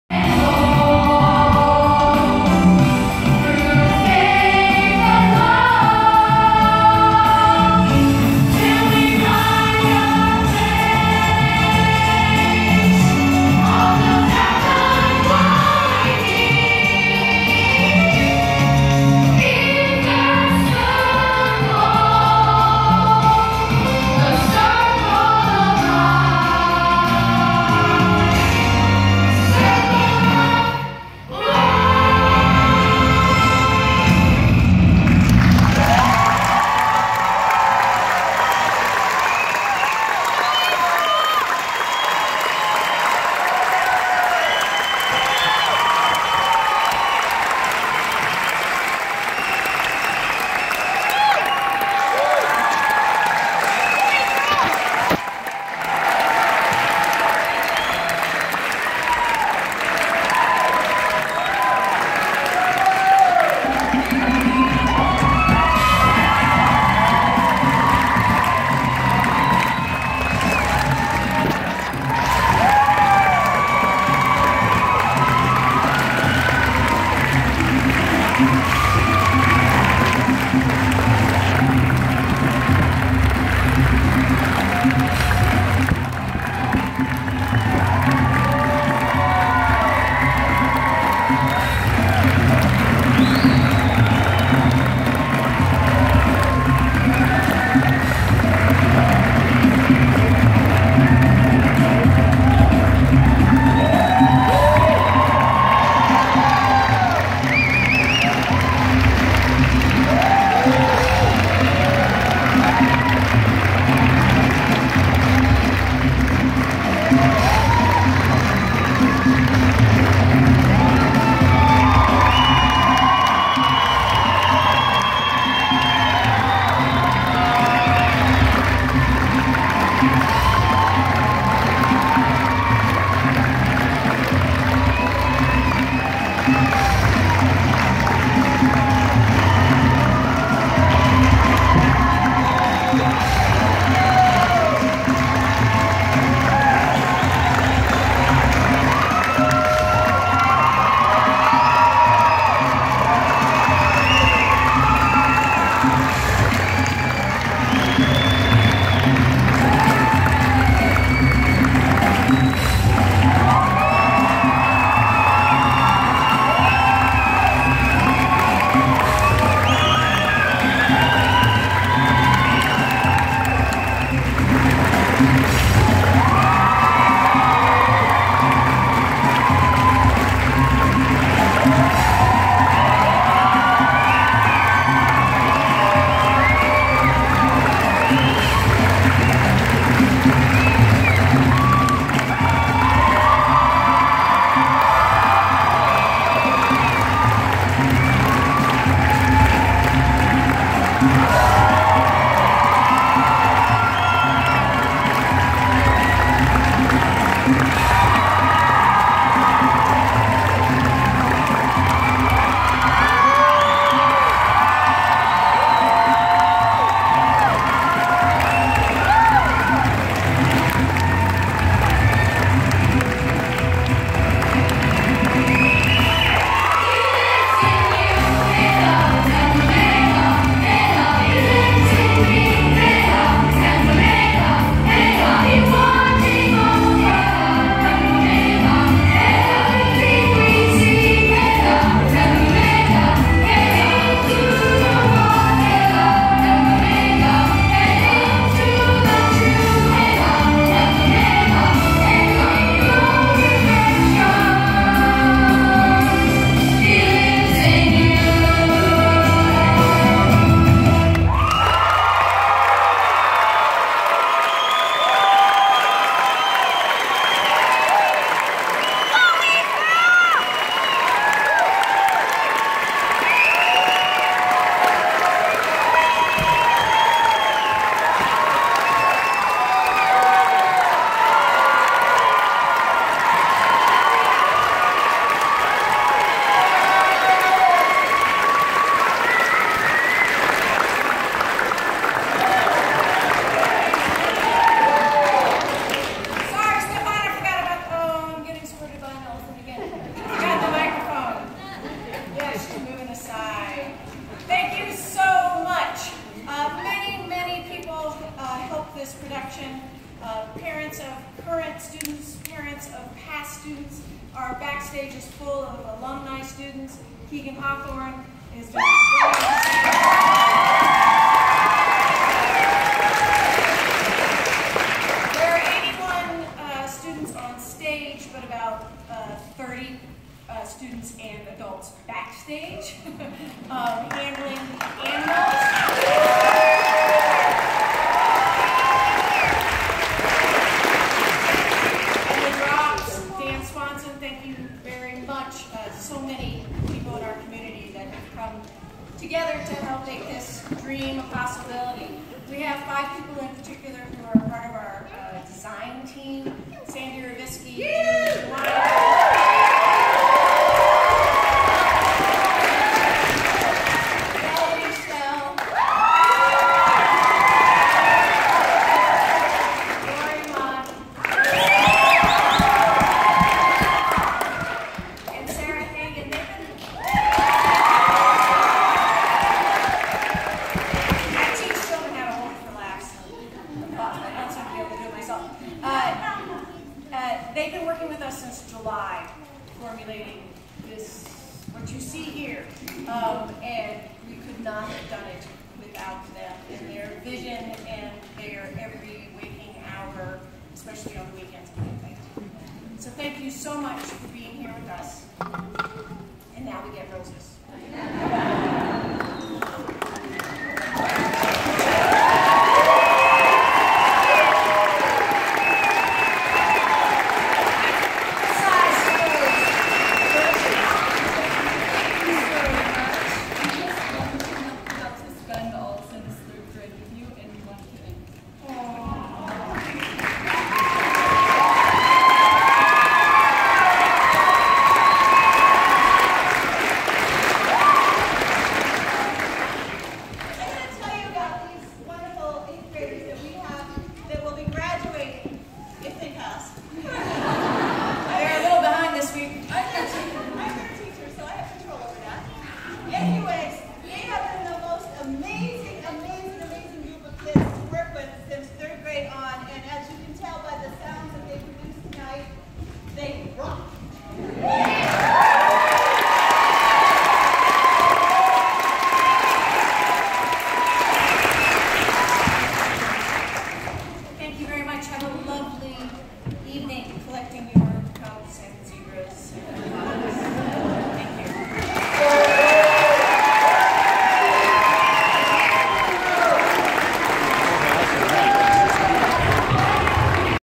Disney's "The Lion King" - Presented by The Little Red Schoolhouse Players
After an amazing performance, here is the curtain call at The Emerson in Bozeman, Montana.  This clip includes comments from the Directors Laurie Kinna and Stephani Lourie.